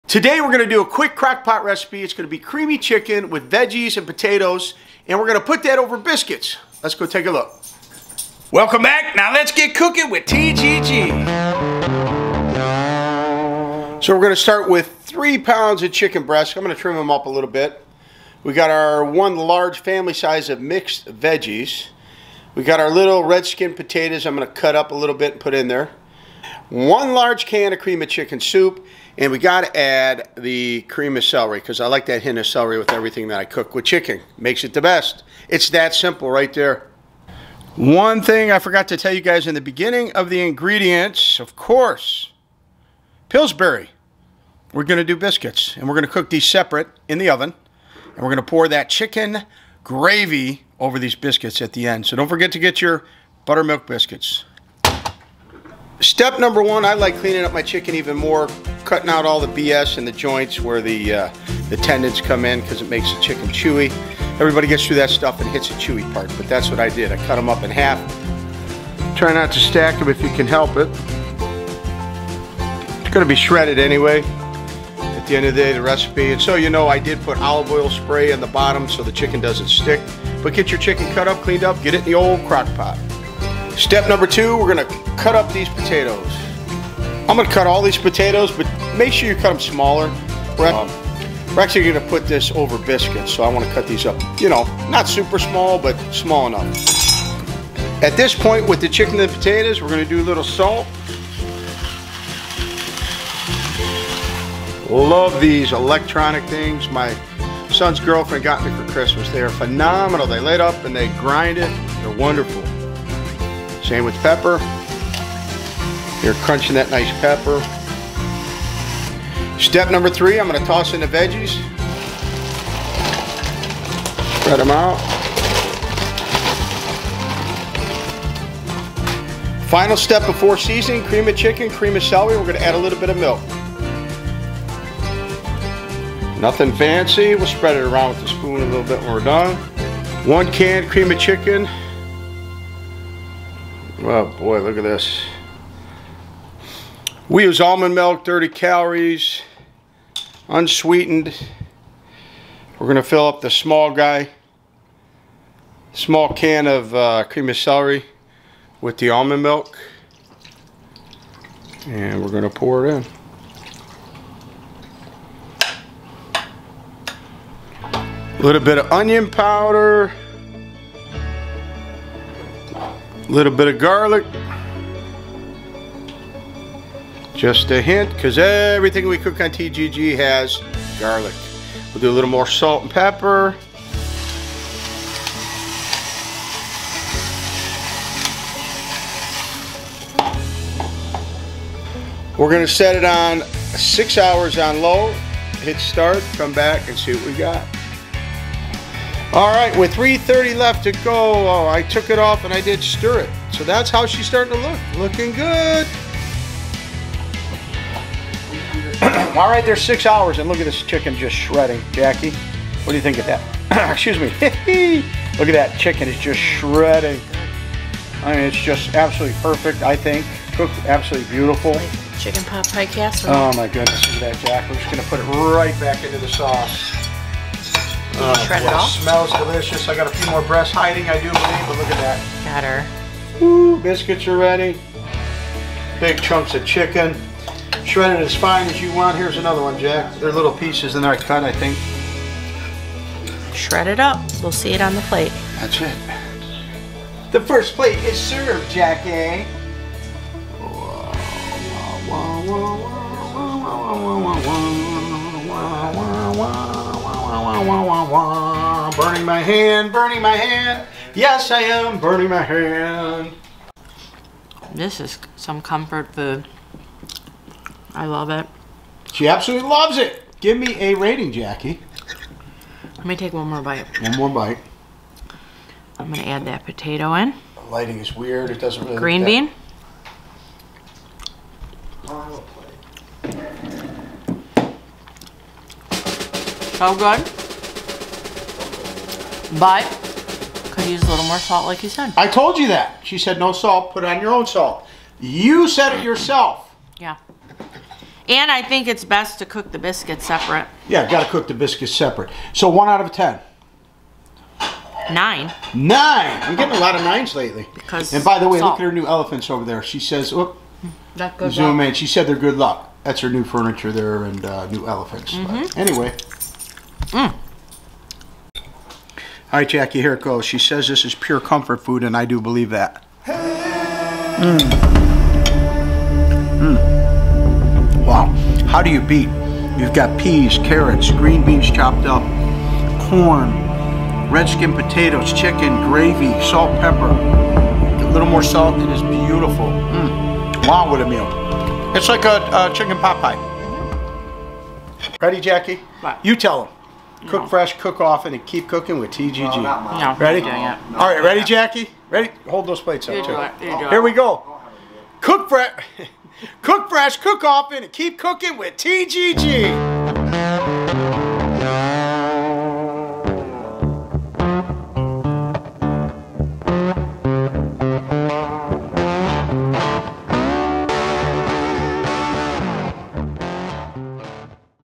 HOW TO MAKE CHICKEN POT PIE
This is a Quick & Easy Crockpot recipe of Creamy Chicken with veggies and red skin potatoes.   Cook on low for 6hrs, shred chicken and serve over frshly baked biscuits.

Only thing I would do different would be to add a packet of dry ranch seasoning!!